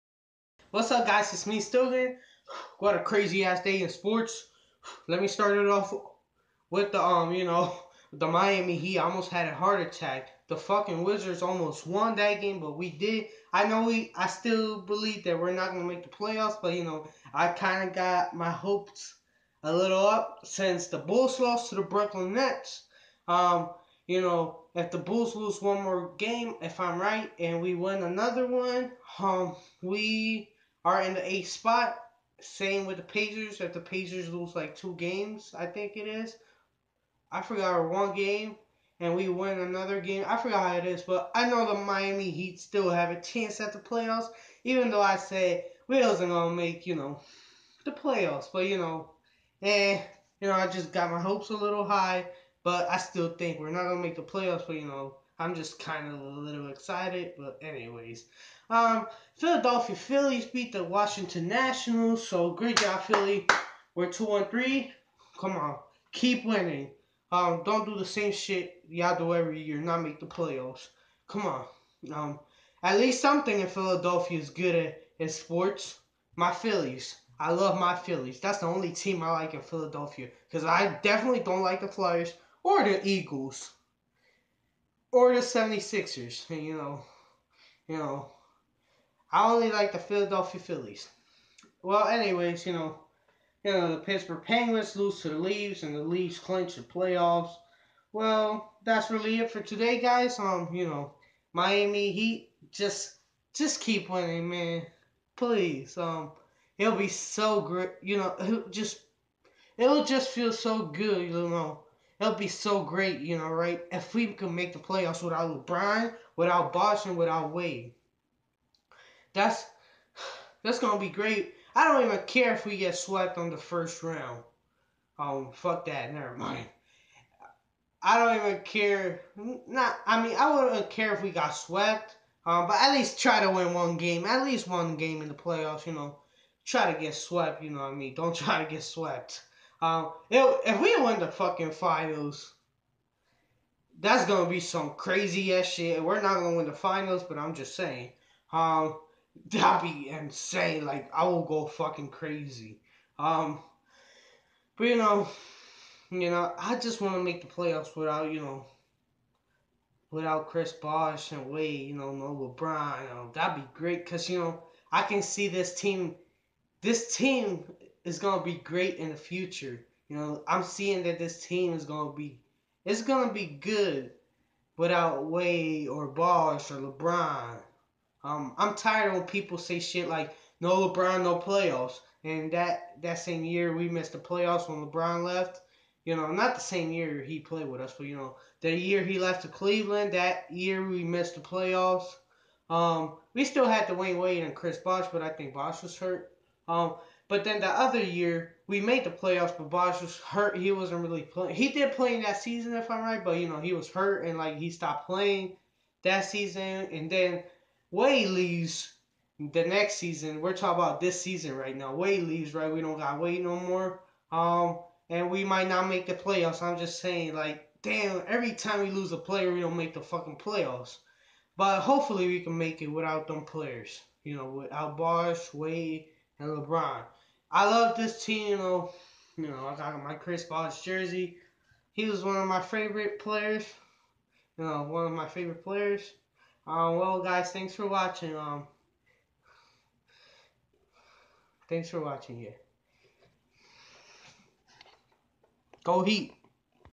Another crazy day in sport's
Basketball: heat beat wizards and bulls lose to nets

Hockey: penguins lose to leafs leafs clinch the playoffs

Baseball: Phillies beat nationals